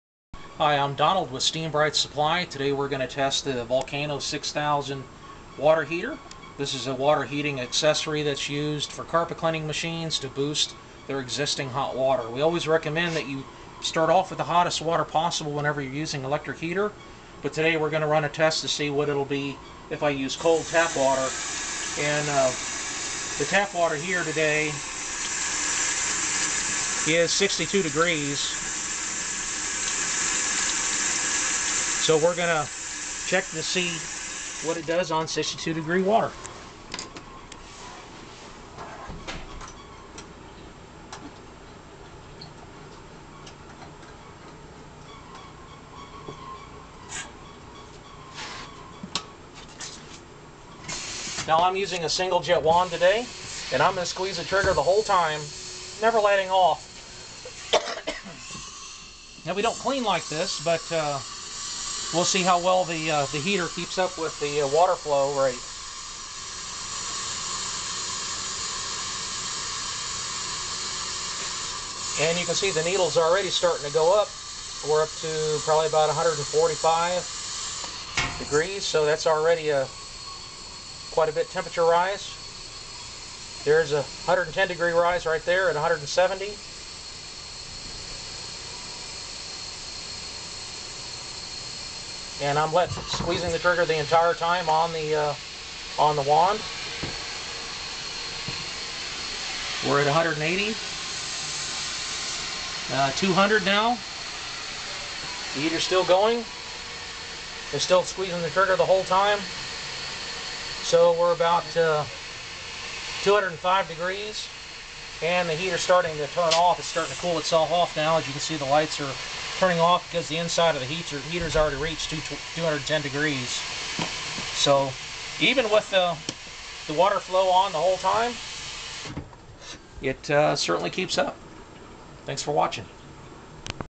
Volcano 6000 Water Heater Performance Test
Donald with Steambrite Supply tests the volcano 6000 watt heater with an incoming water temperature of 62°F and a final resulting output temperature of 205°F (which is where the safety cutoff is for the internal thermostat) while squeezing the valve on the wand, requesting water the entire time. That is over a 140° temp rise!